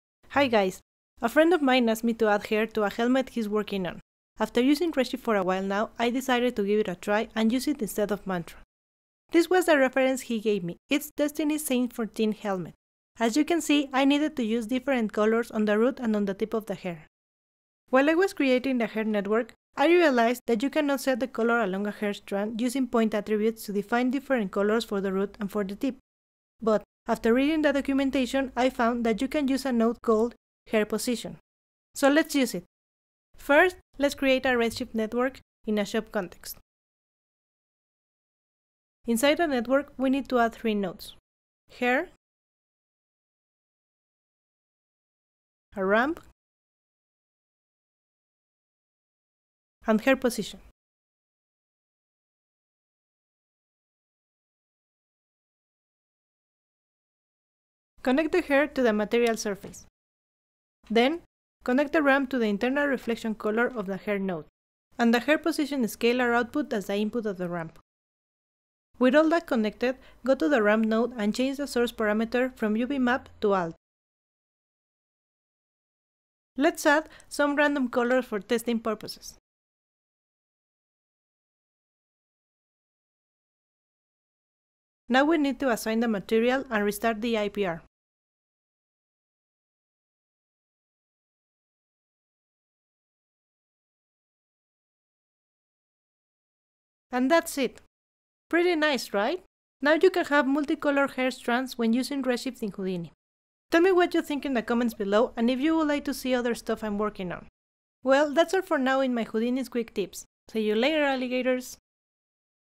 Add Different Colors along Hair Strands in HOUDINI
Learn how to add multicolored hair strands using Redshift in HOUDINI